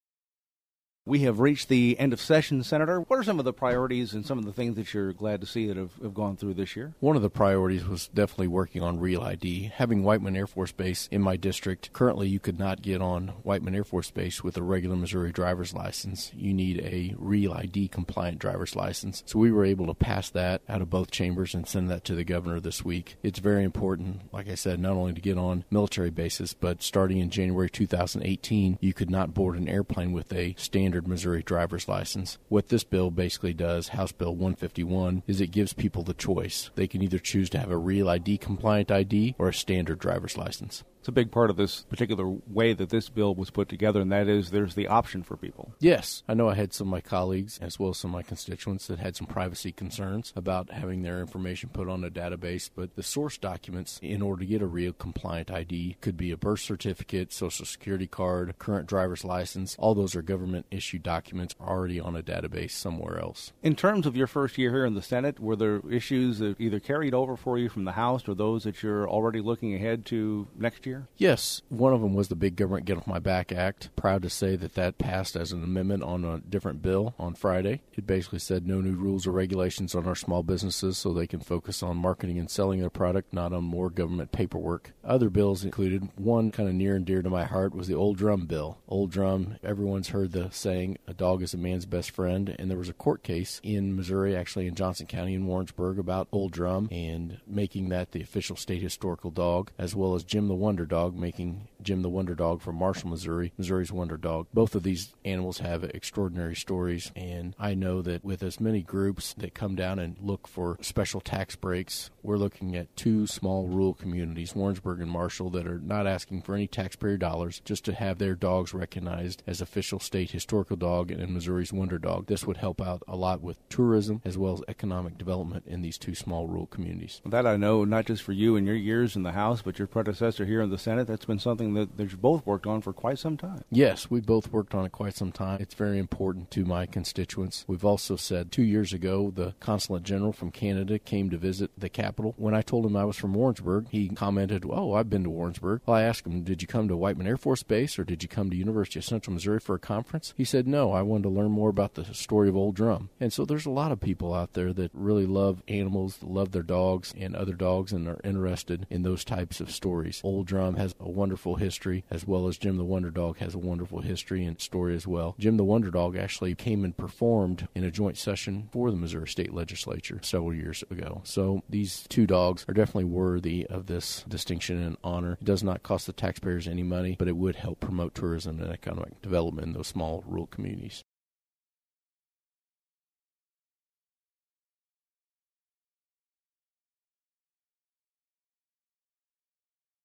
Senator Denny Hoskins' Podcast for the Week of May 8
JEFFERSON CITY — State Sen. Denny Hoskins, R-Warrensburg, discusses his thoughts on the just-completed First Regular Session of the 99th General Assembly, which wrapped up on May 12.